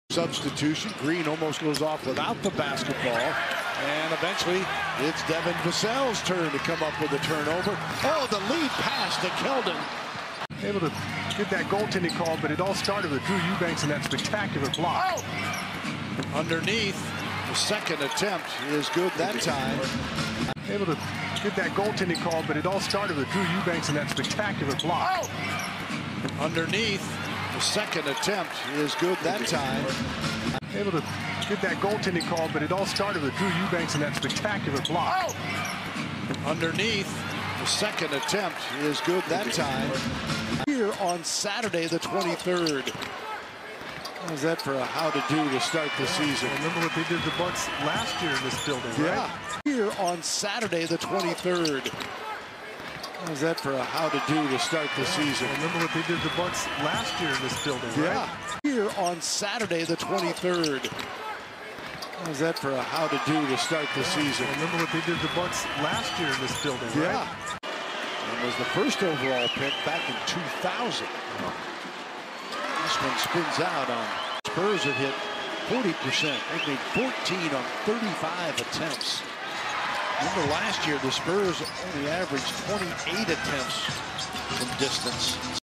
Kenyon Martin Jr. : All Possessions (2021-10-15)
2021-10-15 San Antonio Spurs vs Houston Rockets: Player Highlights Kenyon Martin Jr.
    2 PTS, 2 REB, 0 AST, 0 STL.
        0 of 0 from 3.
        1 of 4 FGs in 9:32 minutes.